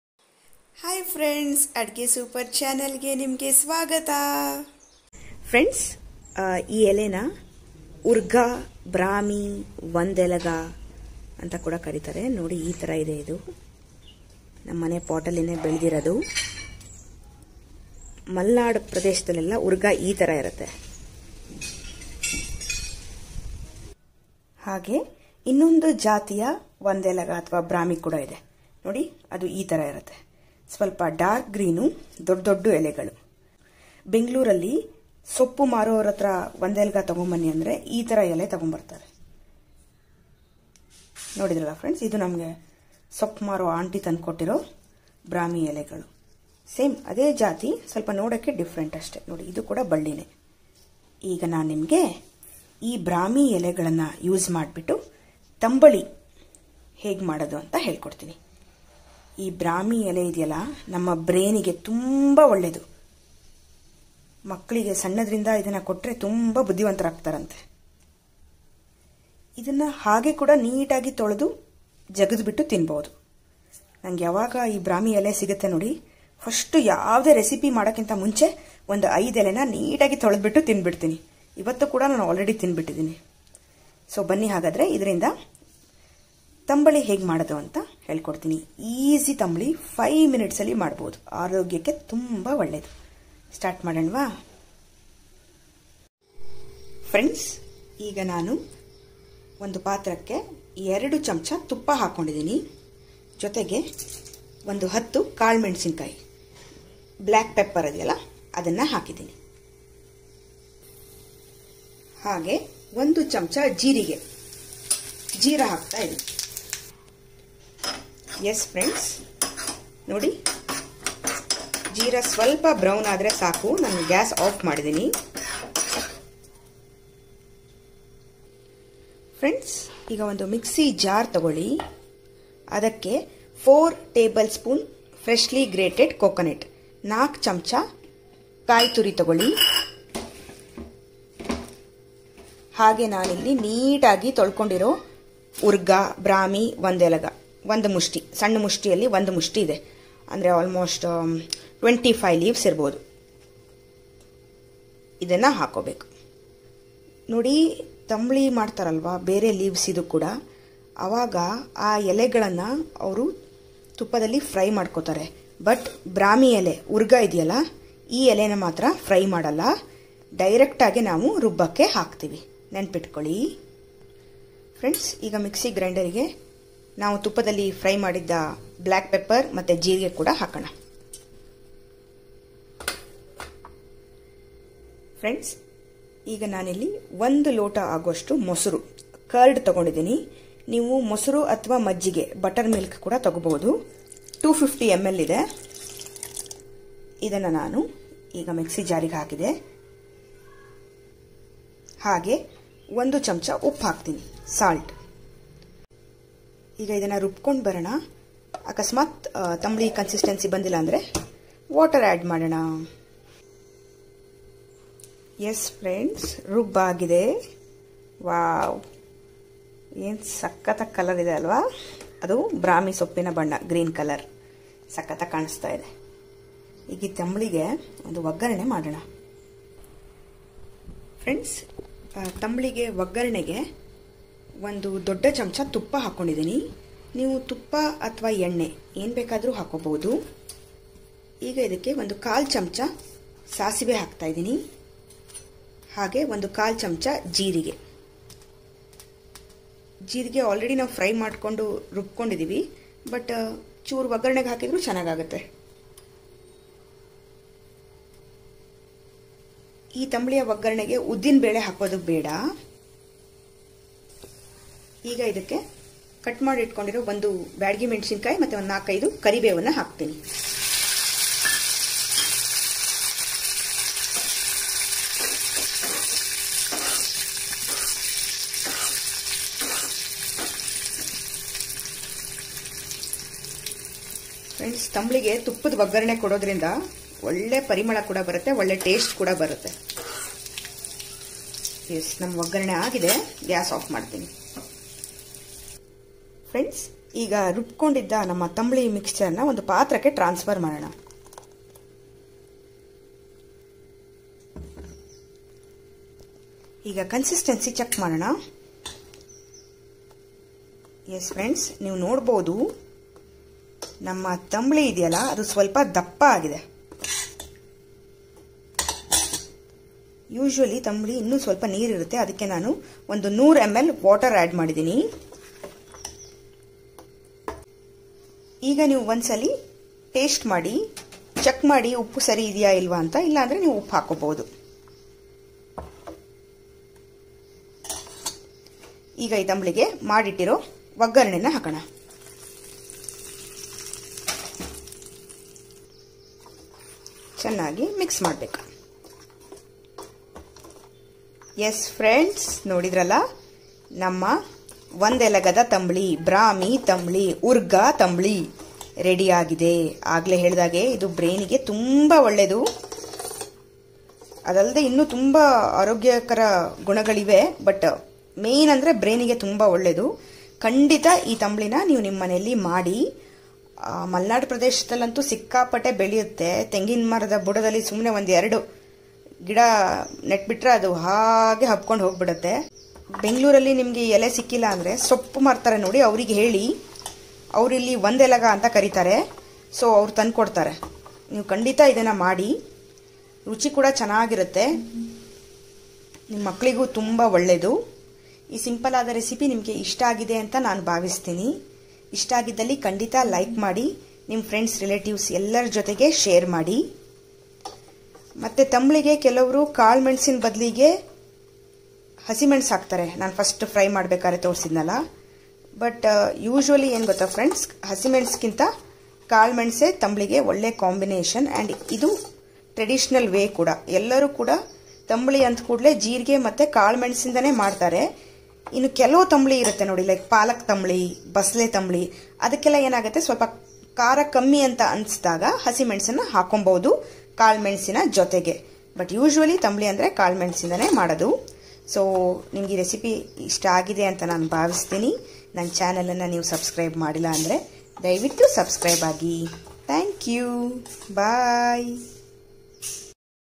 Memory booster Brahmi tambuli/Ondelaga tambuli/Thimare /Urga Tambuli/ಜ್ಞಾಪಕ ಶಕ್ತಿ ಹೆಚ್ಚಿಸುತ್ತದೆ
Brahmi tambuli is the best for your brain.
It helps as a memory booster!

Memory booster Brahmi tambuli/Ondelaga tambuli/Thimare Tambuli/Urga Tambuli/ಜ್ಞಾಪಕ ಶಕ್ತಿ ಹೆಚ್ಚಿಸುತ್ತದೆ